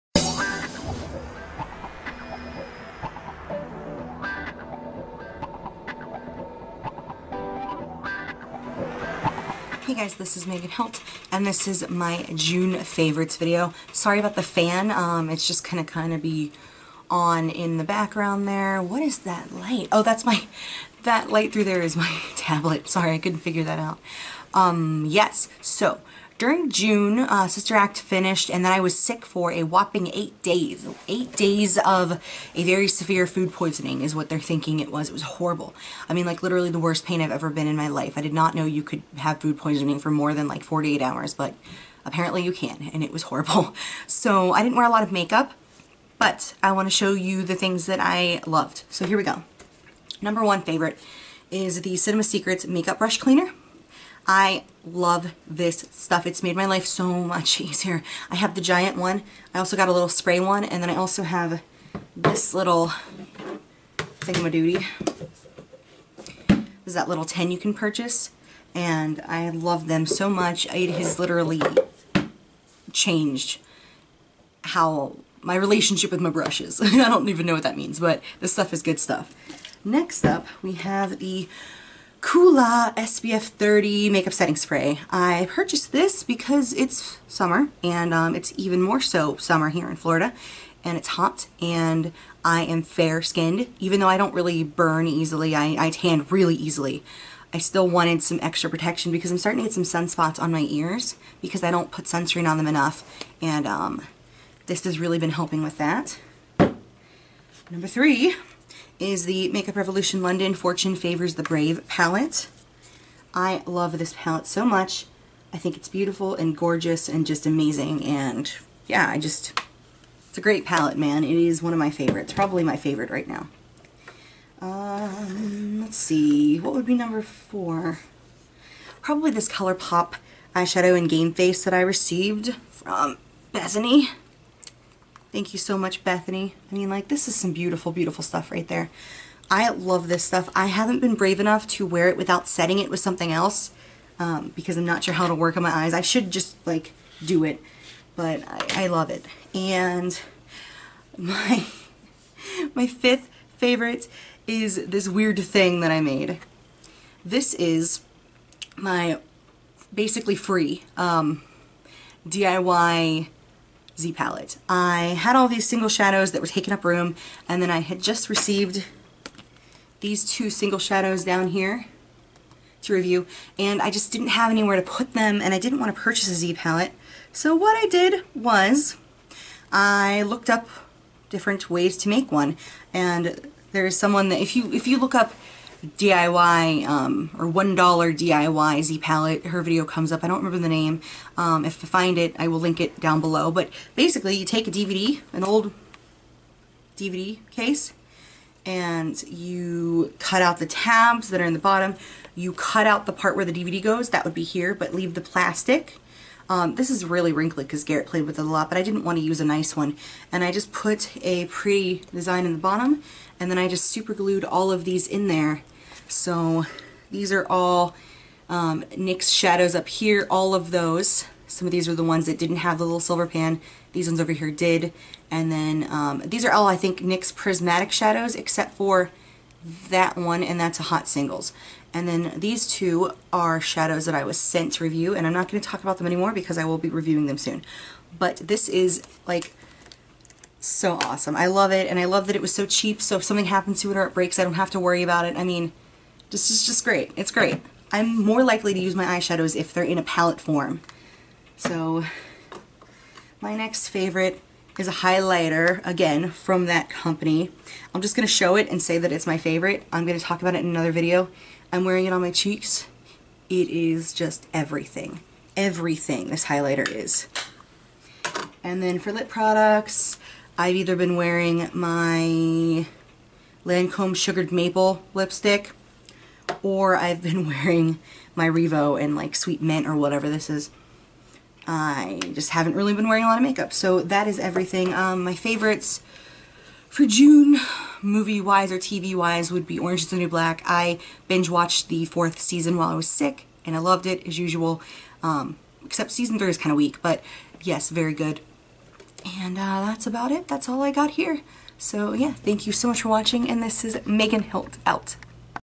June 2016 Favorites
June was a crazy month for me... We finished Sister Act on June 12th and I finished my first week of Summer Classes at OCT. Then on June 17th, I woke up at 3am in excruciating pain. I was sick for around eight days. I was in the Emergency Room twice and lost 30 lbs. It was not fun. They are still not completely sure what was wrong with me.